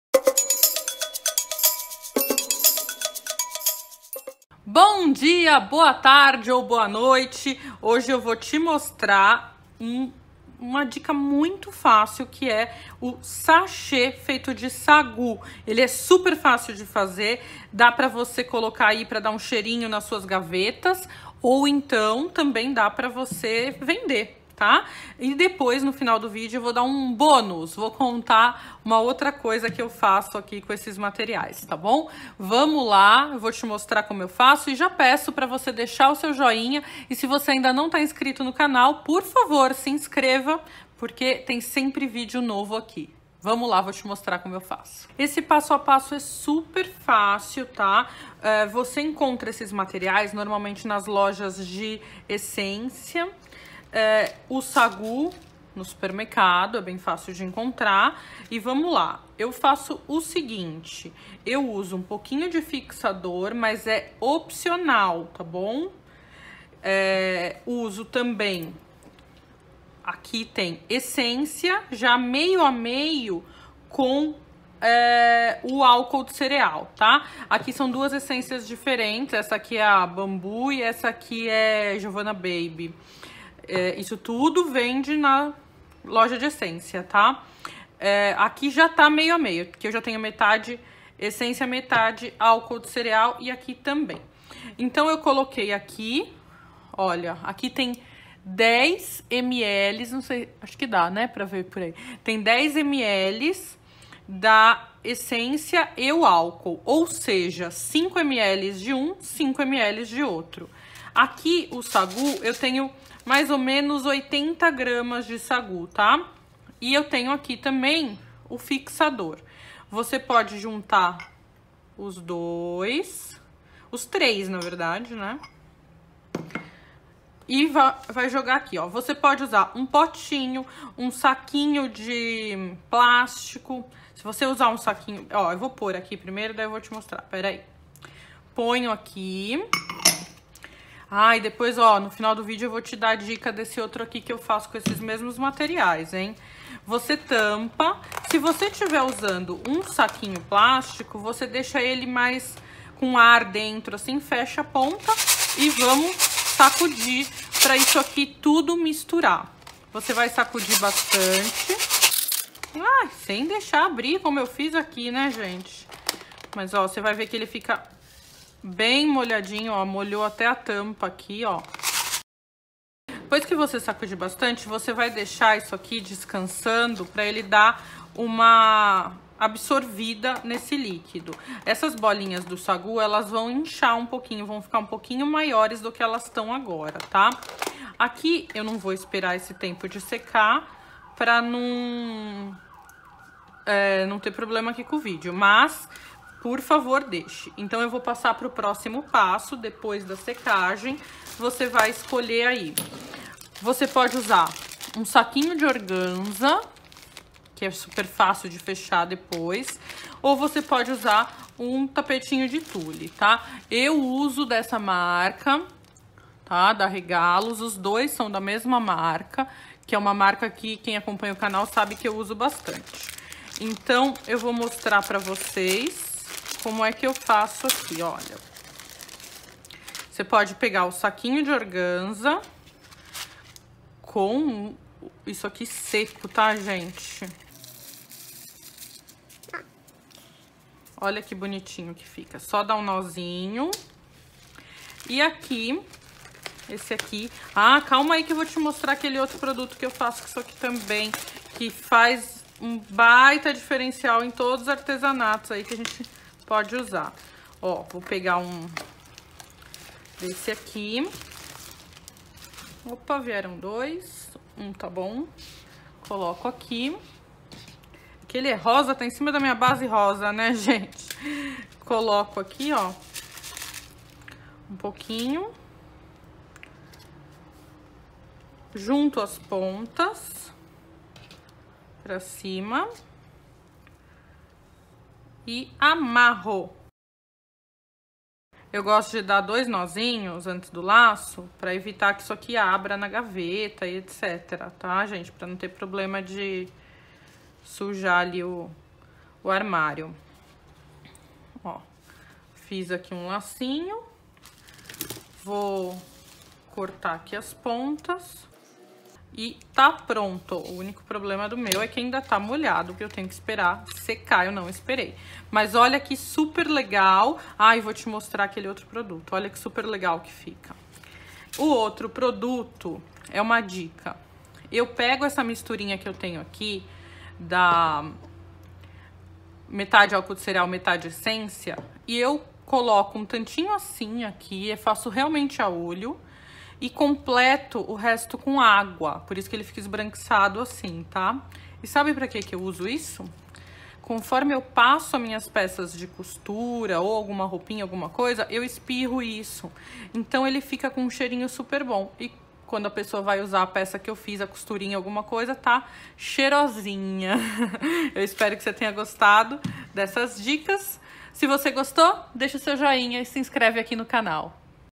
Como fazer SACHÊ com sagú - SUPER FÁCIL
Veja como fazer sachê de sagú. Super fácil de fazer, cheiroso e ideal para suas gavetas, armários ou para presentear e vender. Veja também: COMO FAZER CHAVEIRO ALÇA DE TECIDO